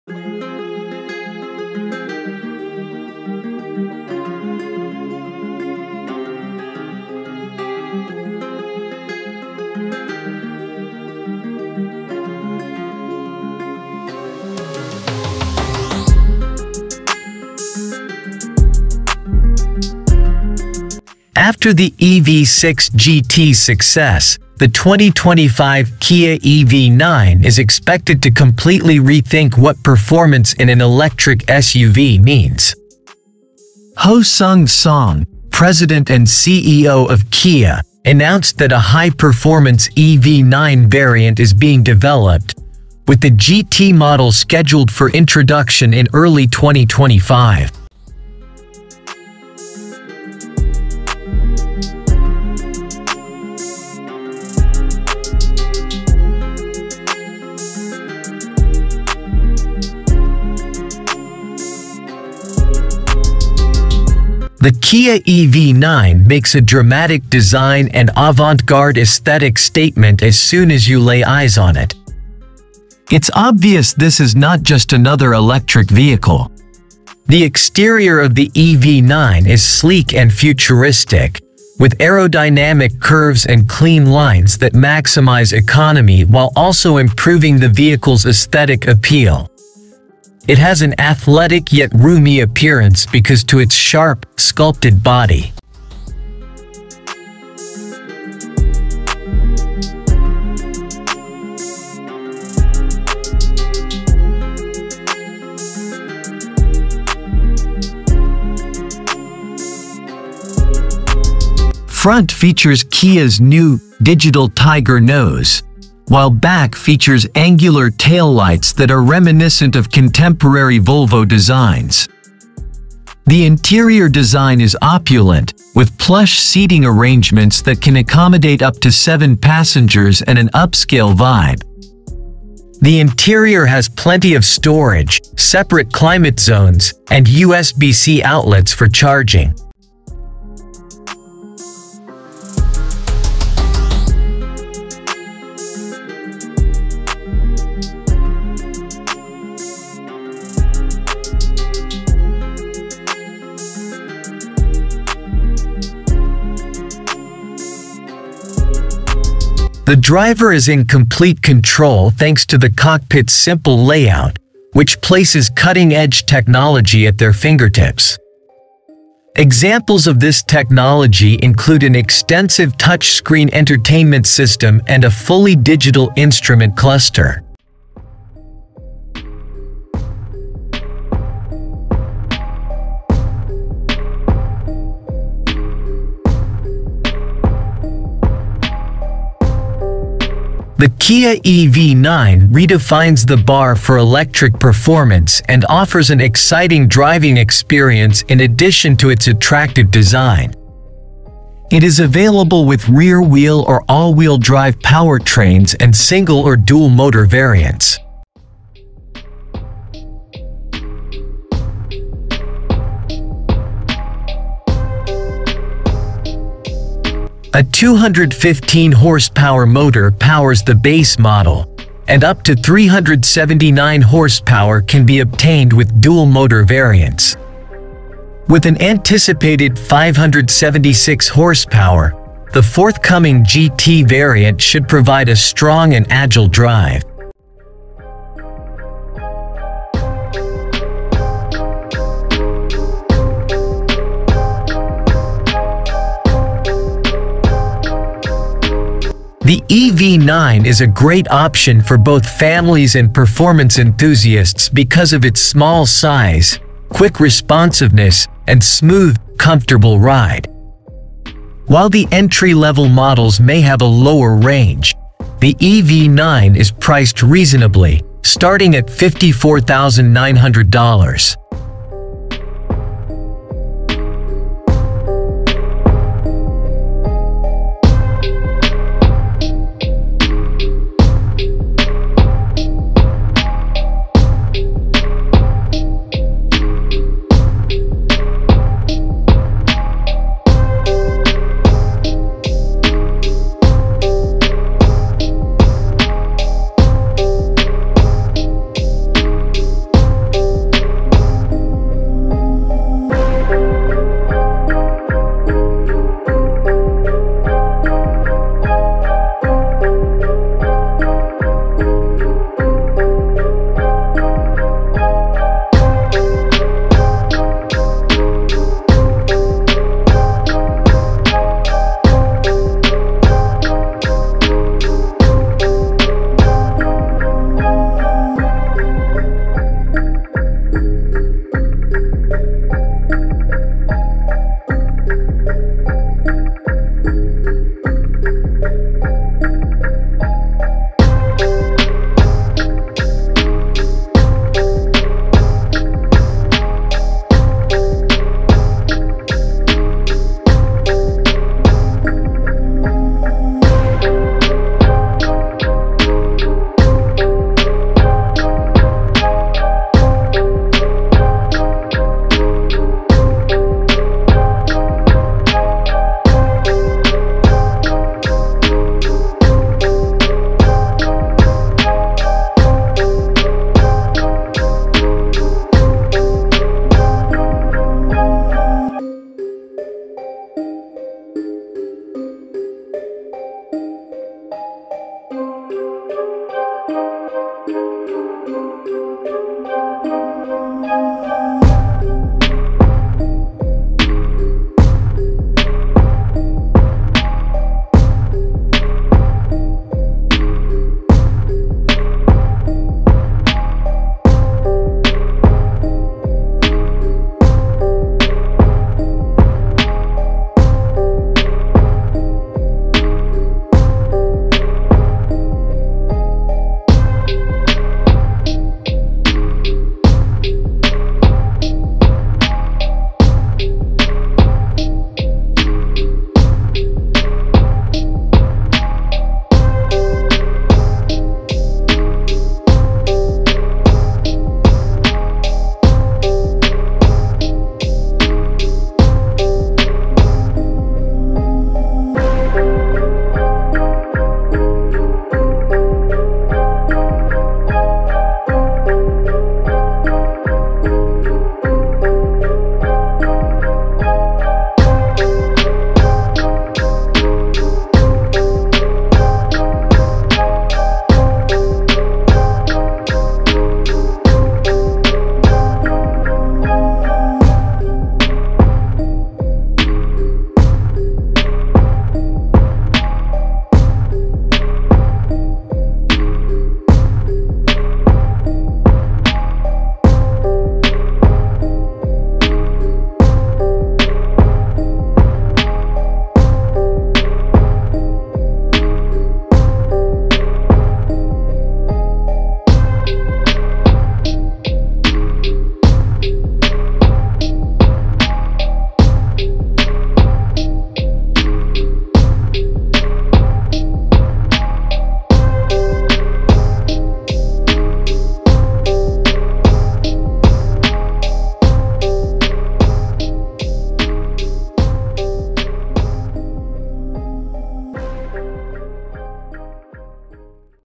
AMAZING.!! 2025 New Kia EV9 - Appears With A Minimalist Design Plus A Spacious And Luxurious Cabin.
#2025 kia kiaev9 electricsuv AMAZING.!! 2025 New Kia EV9 - Appears With A Minimalist Design Plus A Spacious And Luxurious Cabin. @BALCARS7